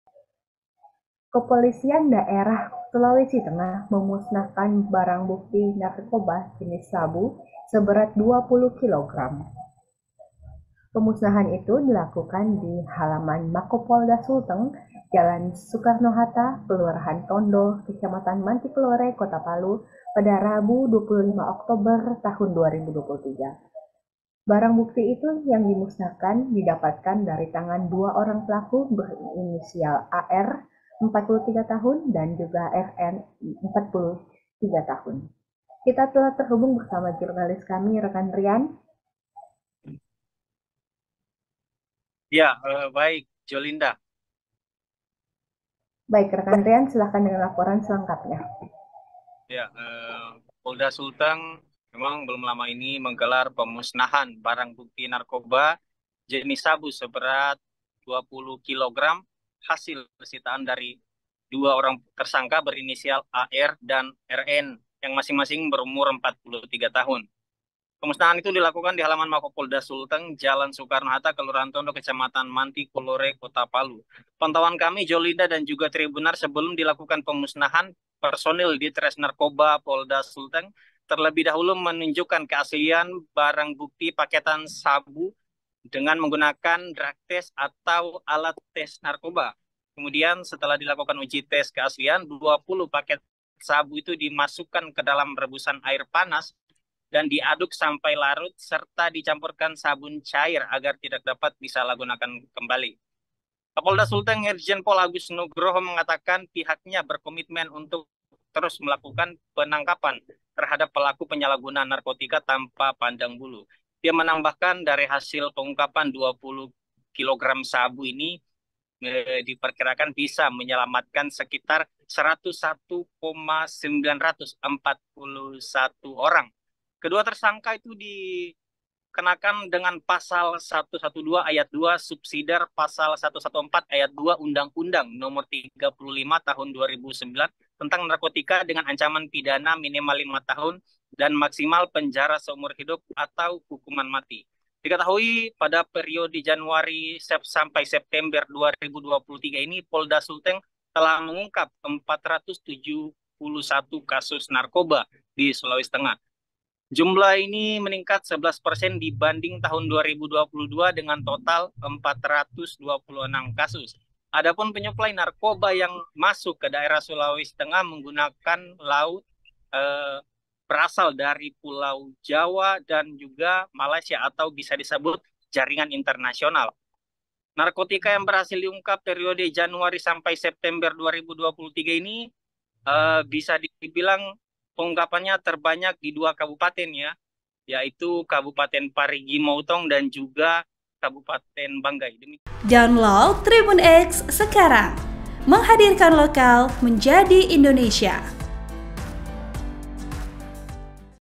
Polda Sulteng Musnahkan 20 Kg Sabu, Pemilik Terancam Penjara Seumur Hidup hingga Hukuman Mati
TRIBUNPALU.COM - Kepolisian Daerah (Polda) Sulawesi Tengah (Sulteng) memusnahkan barang bukti narkoba jenis sabu seberat 20 kilogram.

Pemusnahan itu dilakukan di hamalan Mako Polda Sulteng, Jl Soekarno Hatta, Kelurahan Tondo, Kecamatan Mantikulore, Kota Palu, Rabu (25/10/2023) siang.

Pemusnahan disaksikan langsung Gubernur Sulawesi Tengah, Rusdy Mastura dan perwakilan TNI mapun kejaksaan mitra Polda Sulteng.

Penulis: Rian Afdhal | Editor: Haqir Muhakir
VP : Sidik

Update TribunPalu.com:
Fanpage Facebook : Trlibunpalu.com
YouTube TribunPalu: Tribun Palu Official